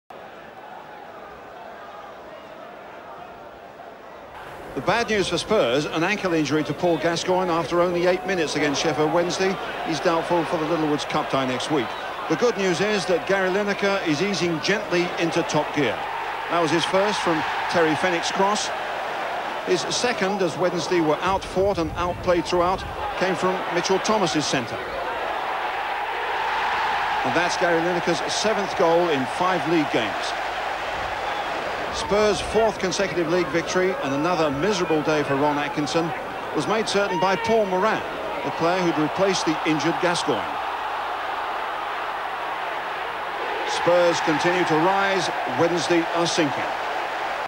Spurs v Sheffield Wednesday 21-10-1989
After a slow start to the season, Spurs have now won their last three. Wednesday remain bottom with just one win.